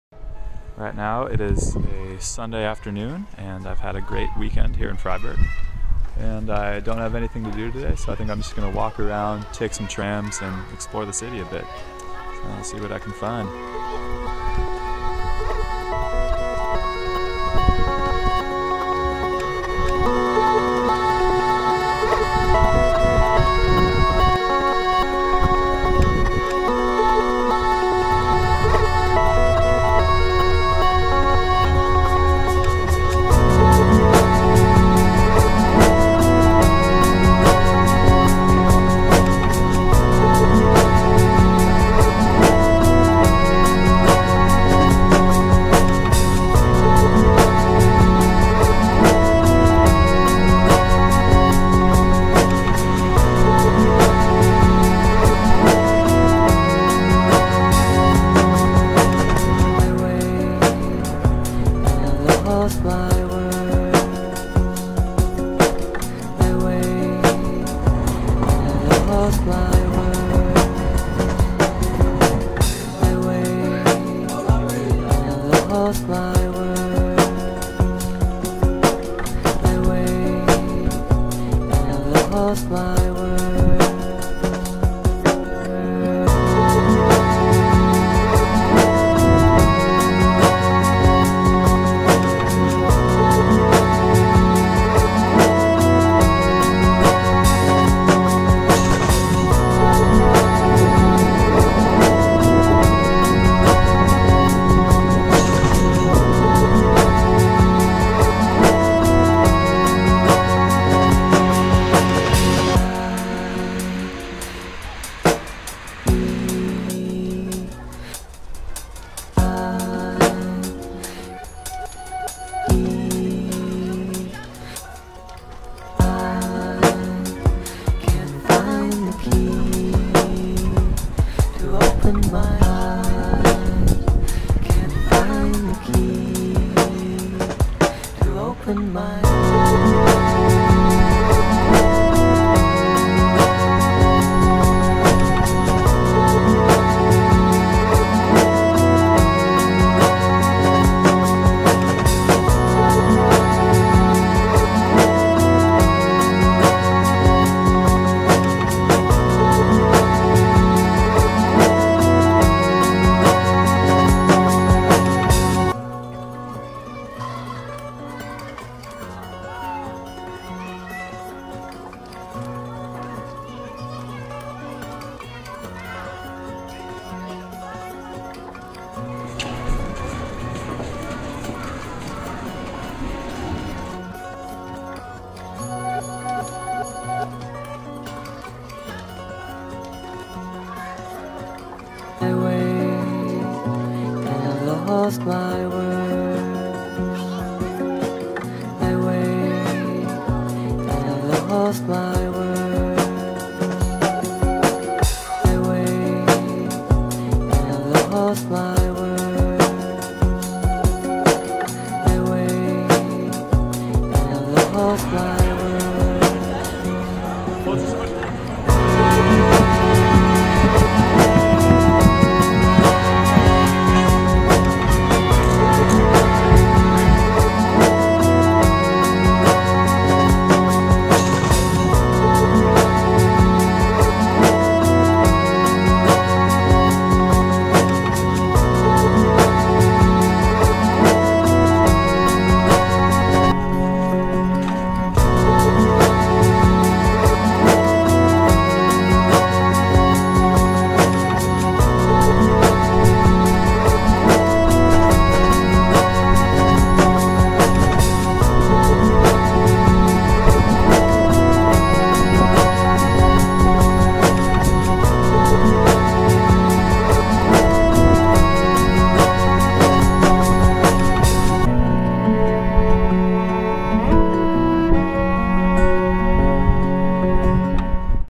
Exploring Freiburg and Staufen
An afternoon in Freiburg and an evening at a carnival in Staufen.